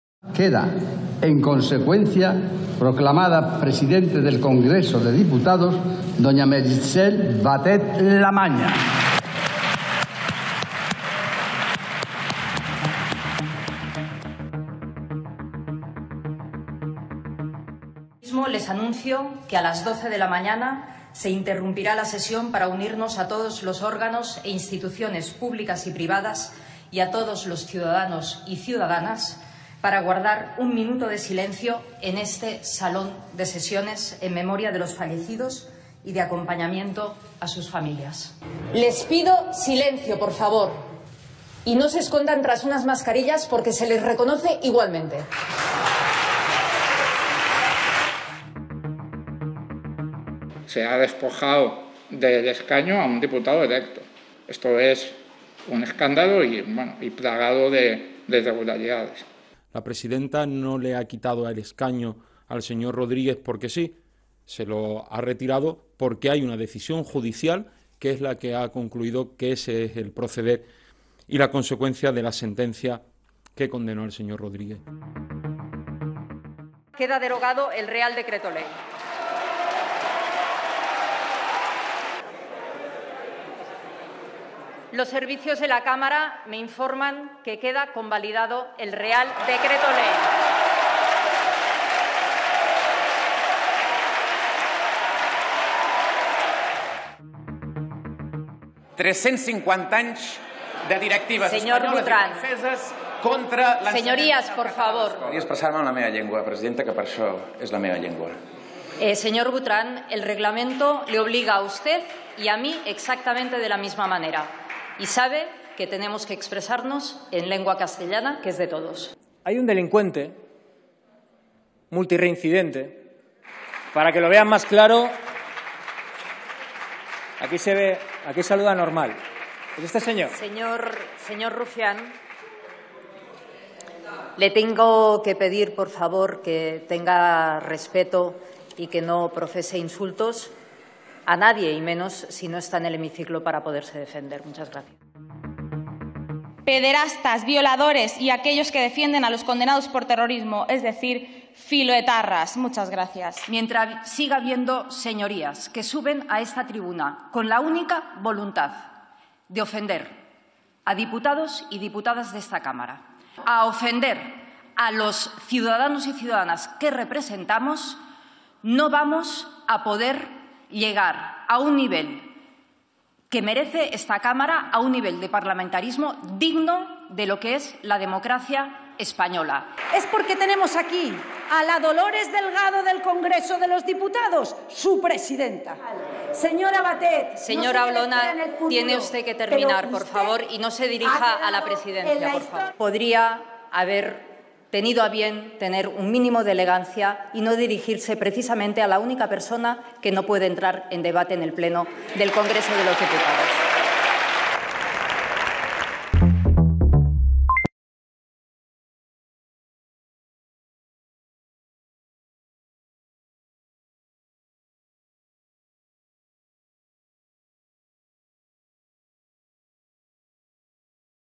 Los cinco momentos más destacados de MERITXELL BATET como presidenta del Congreso | EL PAÍS
“Les ruego encarecidamente que se comporten con más respeto y más educación, y que abandonen los insultos y las ofensas”. Esta es una de las llamadas de atención que la presidenta del Congreso de los últimos cuatro años, Meritxell Batet (PSOE) lanzaba, de forma repetida, en las sesiones de control al Gobierno en la pasada legislatura. La política socialista ha anunciado que no se postulará de nuevo al cargo por motivos personales. La renuncia se produce a una semana de la constitución del nuevo Congreso que será el próximo 17 de agosto y donde el PSOE deberá buscar otro candidato para ese puesto.

Este vídeo es un recorrido por los cuatro años en los que Batet ha ejercido como tercera autoridad del Estado. Desde presidir un Congreso en plena pandemia de la Covid-19 hasta la suspensión del diputado de Podemos, Alberto Rodríguez, pasando por la ‘inédita’ votación de la reforma laboral. La política catalana ha vivido en primera persona una legislatura parlamentaria muy convulsa caracterizada por la crispación y los momentos de tensión en la Cámara Baja.